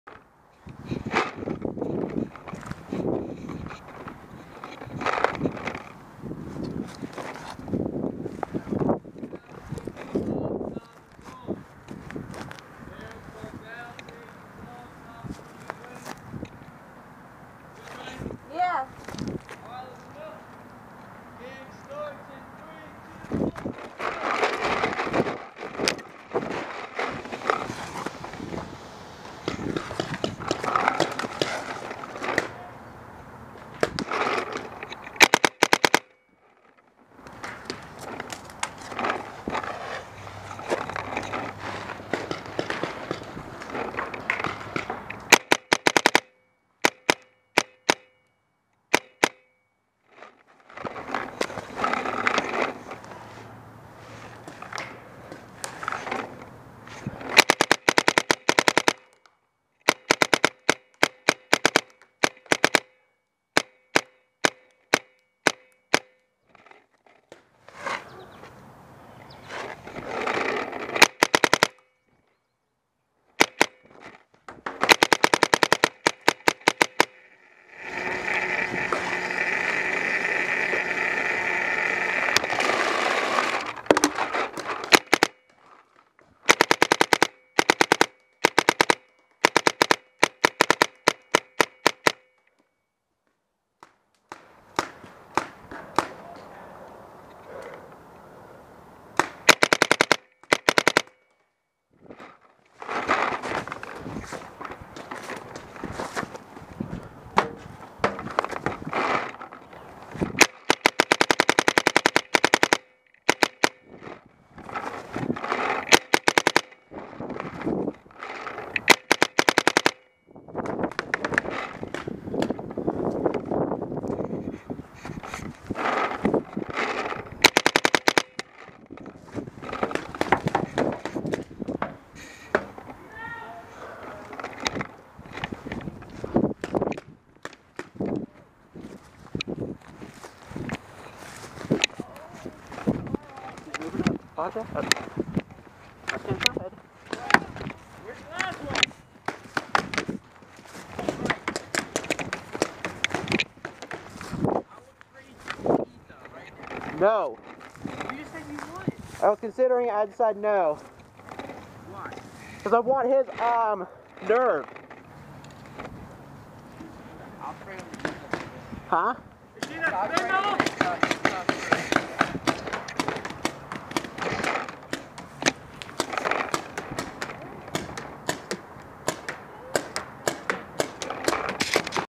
Baltimore Anarchy practice #3 @ Route 40 Paintball
this is Baltimore Anarchys 3rd official practice at route 40 paintball. my setup is a etek 3 a dye rotor with a virtue crown sf and a empire 68/4500 and a shaft 4 barrel kit. Shoutout to Ms.Mimi shes the one who inspires us all to achieve great things!